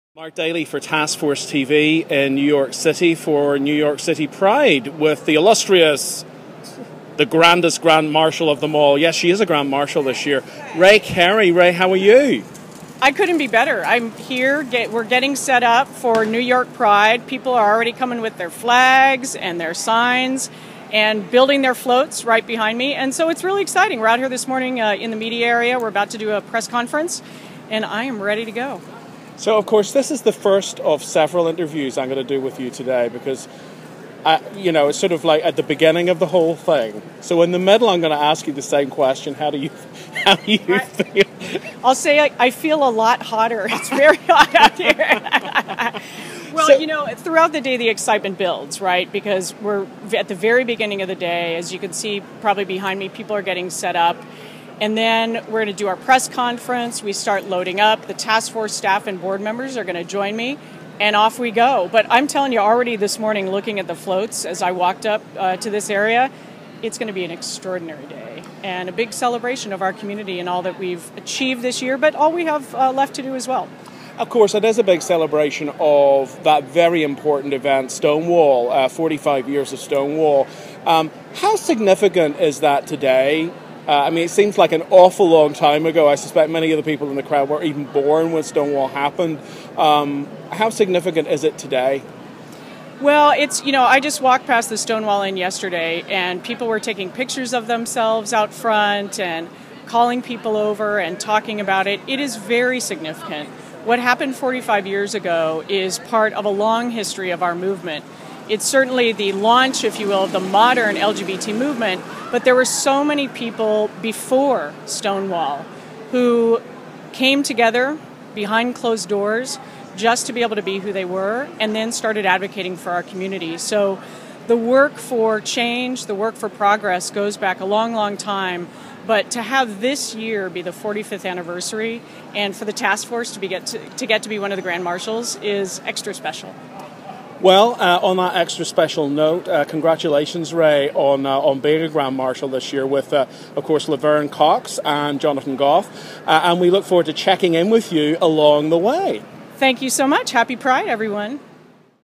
Grand Marshal Rea Carey interviewed at NYC Pride
NYC Pride Grand Marshal and Executive Director of the National Gay and Lesbian Task Force interviewed by Mark Daley of Task Force TV